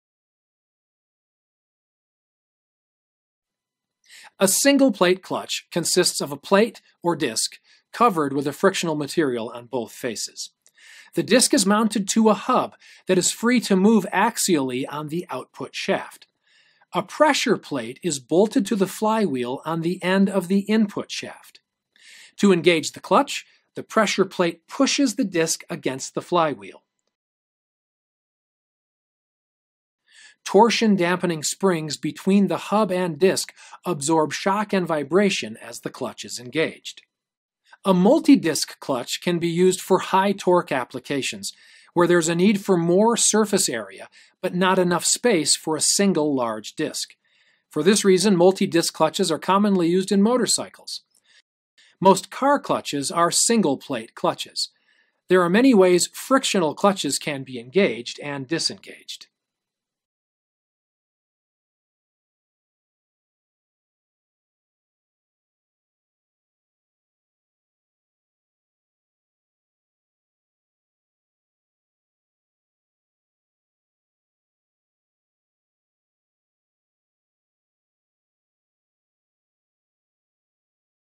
Clutch Basics
to view the full video and purchase access to our other Industrial Maintenance courses.

A clutch is a device that connects and disconnects a driving (input) shaft and a driven (output) shaft. Clutches are important for applications where the motor needs to be started before it is connected to the load or disconnected before it is stopped. There are many different types of clutches, each with advantages and disadvantages. The proper choice depends on the speed, torque, loading characteristics, and operating temperature of the application.